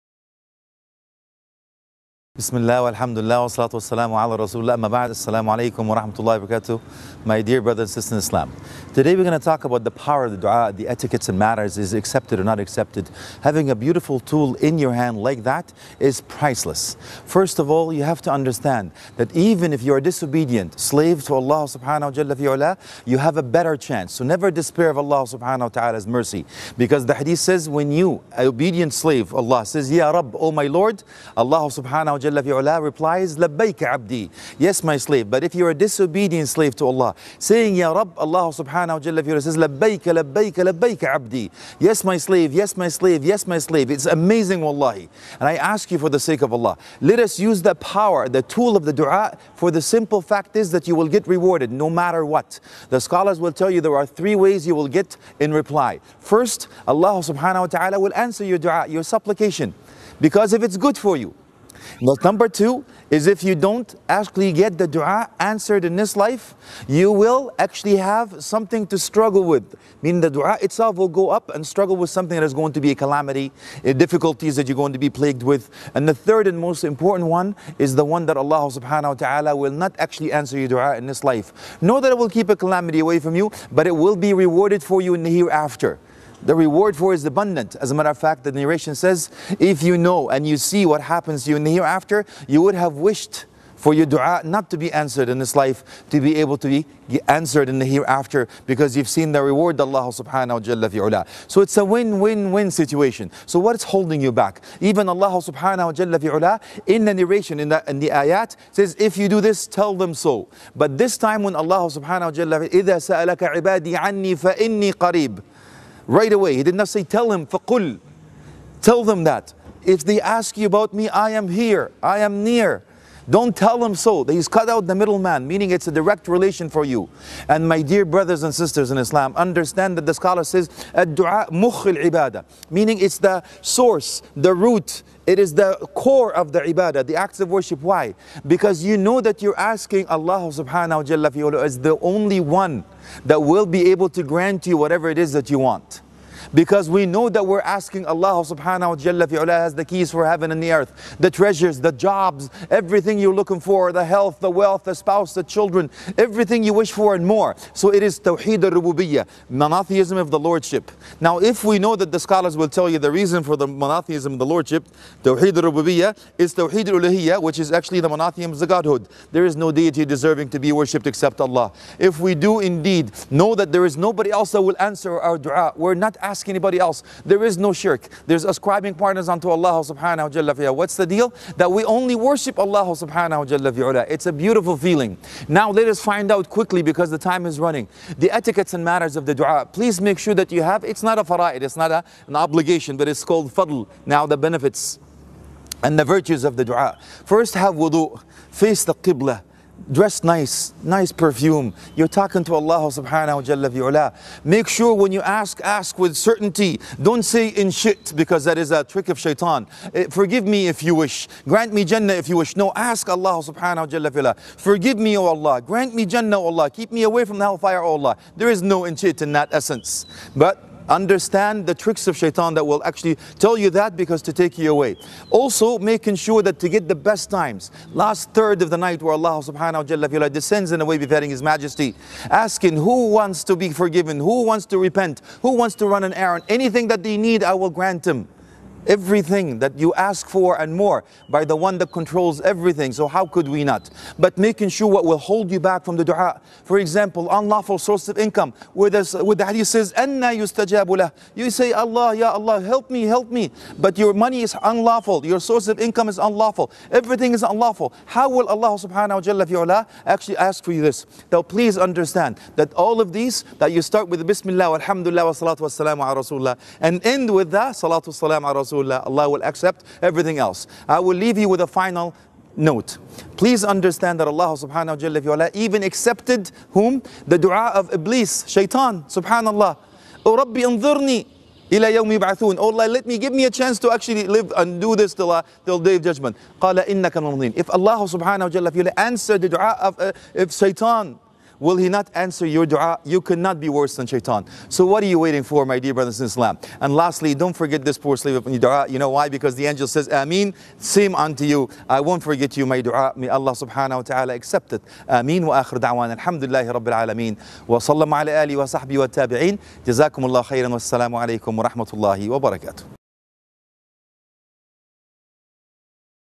Ramadan Reminders: The Power of Dua - Sheikh Alaa Elsayed
Welcome to our Ramadan Reminders series.  A series of beneficial reminders to uplift and educate throughout this blessed month.

Special thanks to AlKauthar Institute for the video